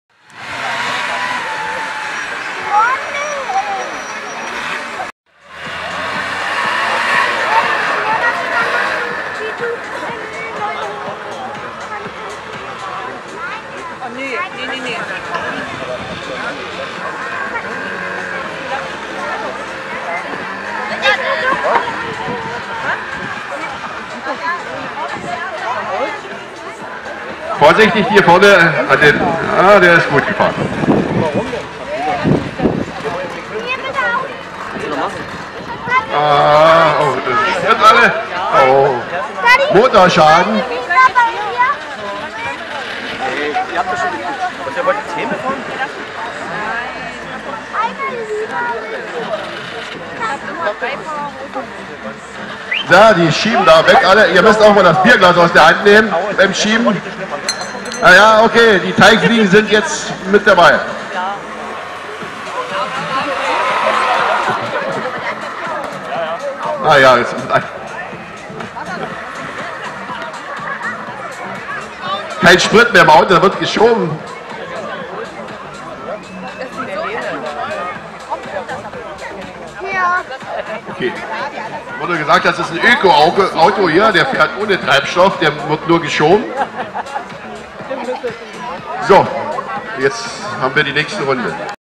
06 Team Teichfliegen vs. Team ?
Die Teichfliegen fahren in der nächsten runde wieder gegen ein Team deren Namen ich wieder nicht herusu gefunden habe.